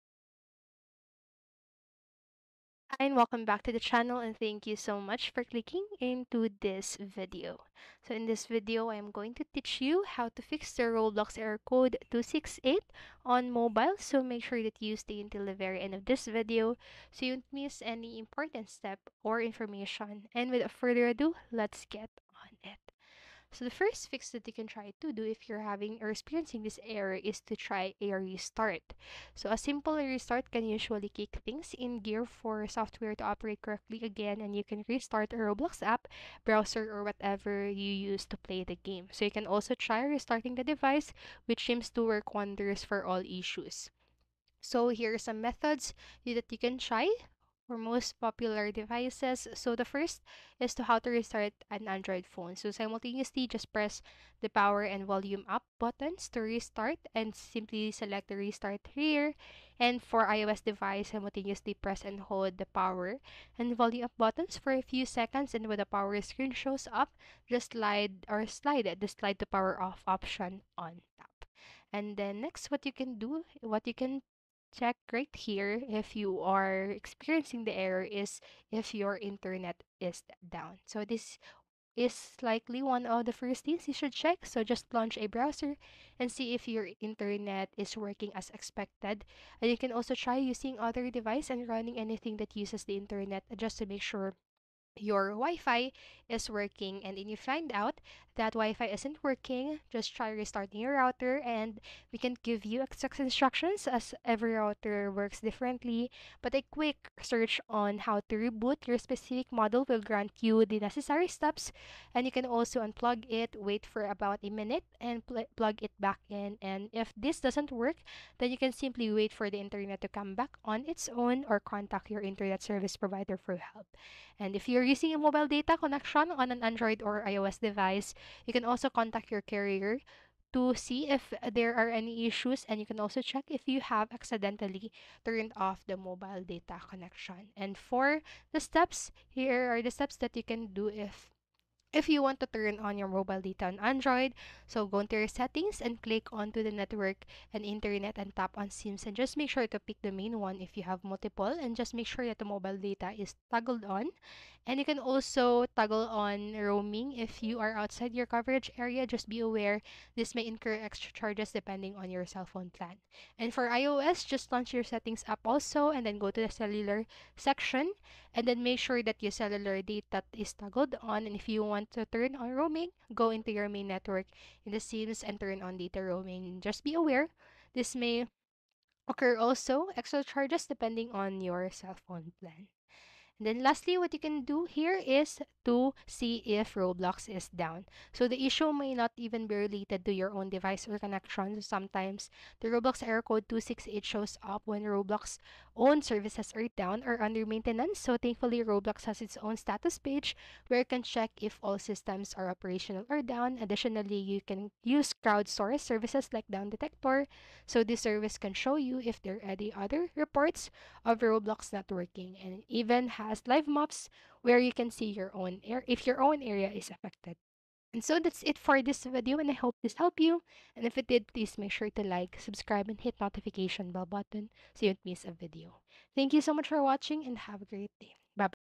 How To Fix Roblox Error Code 268 On Mobile (2024 Guide)
Learn How To Fix Roblox Error Code 268 On Mobile. This video will show you how to fix error code 268 roblox. If you're wondering how to fix roblox error code 268 in mobile this is for you.